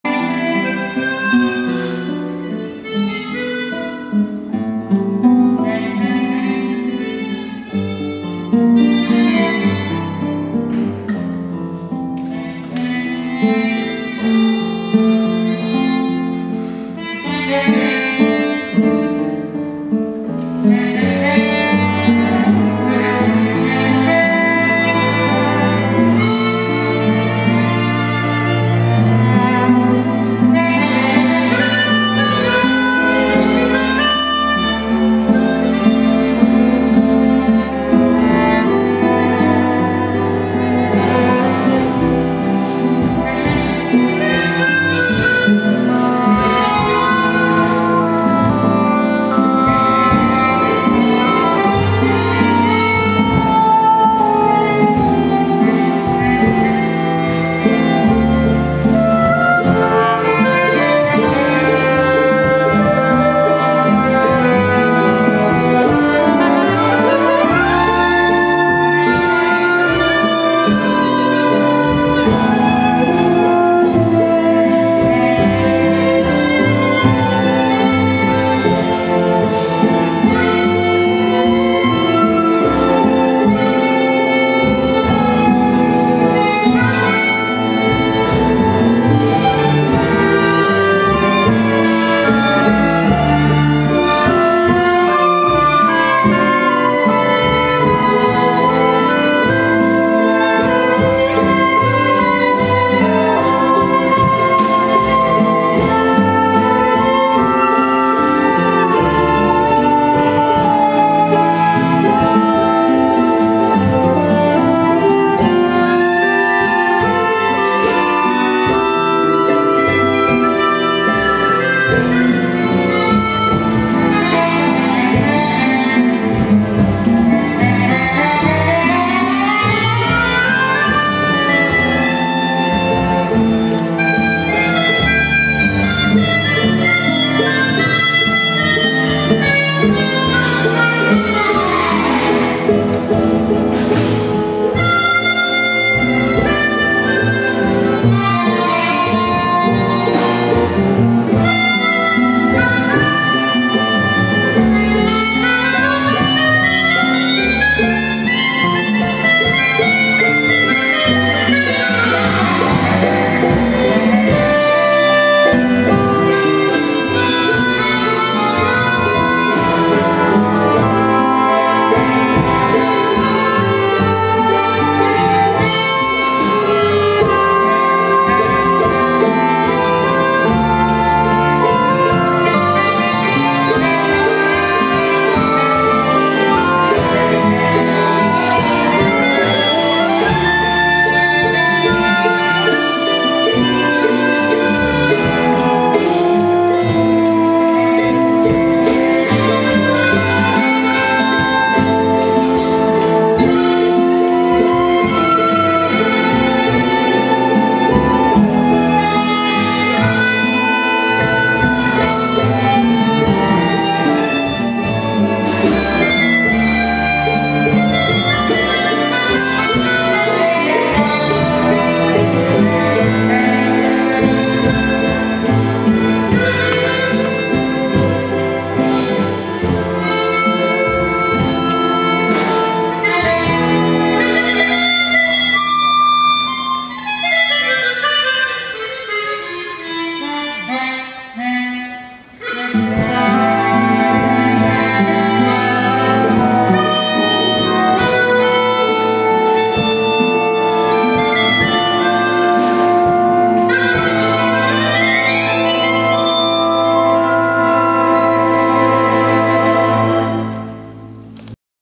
OSRJ - Cartola com Rildo Hora
Tributo a Cartola - Sala Cecília Meirelles (2008)
Orquestra de Solistas do Rio de Janeiro
Convidado Especial: Rildo Hora
Ensaio filmado por Juliano Barbosa